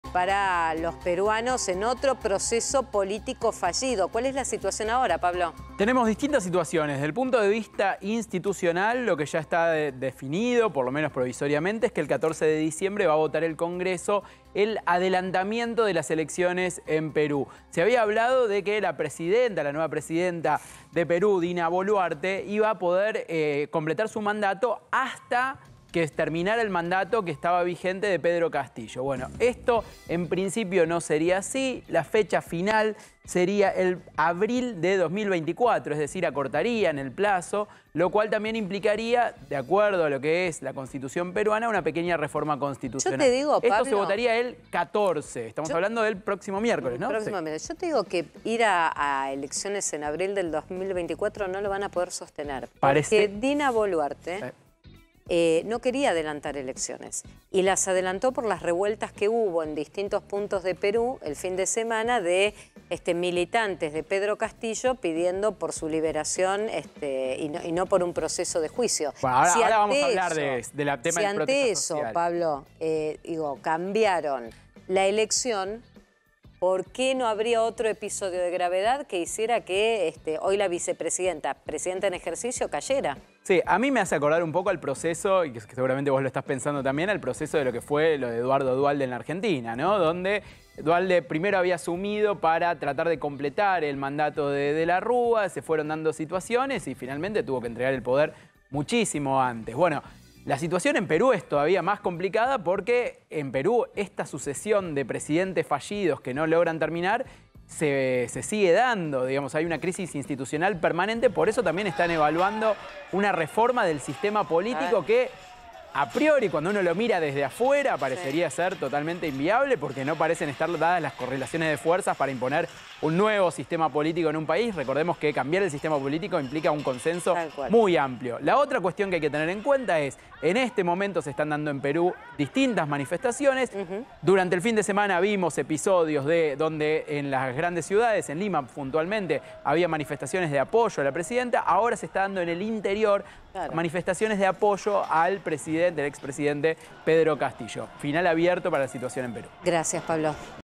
🔴 Crisis política en Perú: pedirán adelantar las elecciones
✋ La presidenta del Perú pedirá para fin de año, el adelanto de las elecciones.

🗣️ Pablo Winokur
👉 Seguí en ElDiarioDeLaTarde